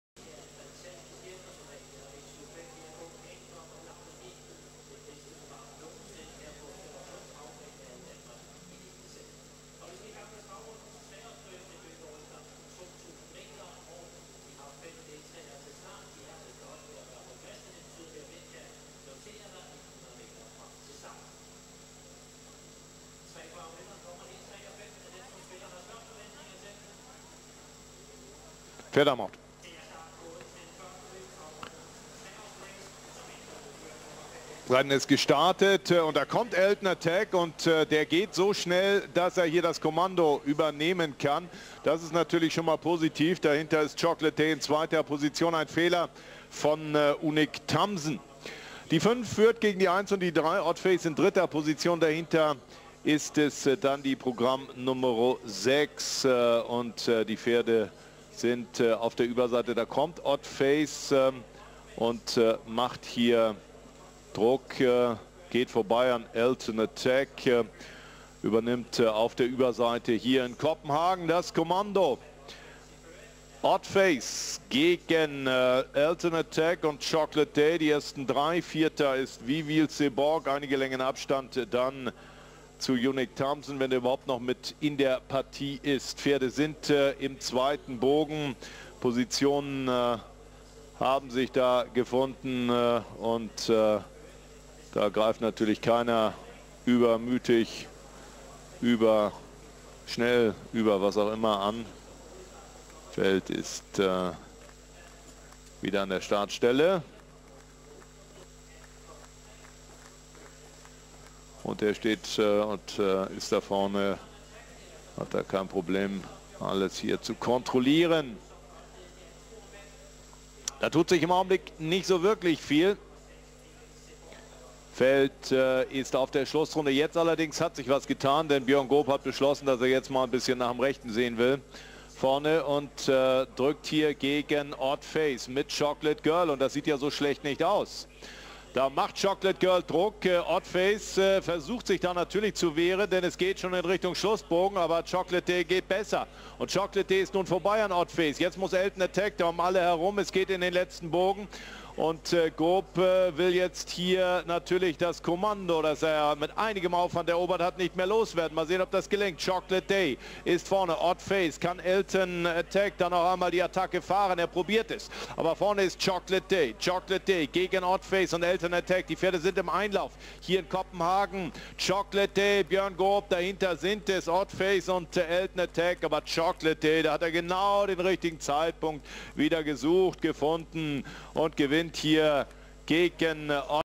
Elton Attack-Derbynennung 2014-3. Platz in Charlottenlund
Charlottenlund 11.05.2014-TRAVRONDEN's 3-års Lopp

1. Chocolate Day* (1) 2. Odd Face (3) 3. Elton Attack (5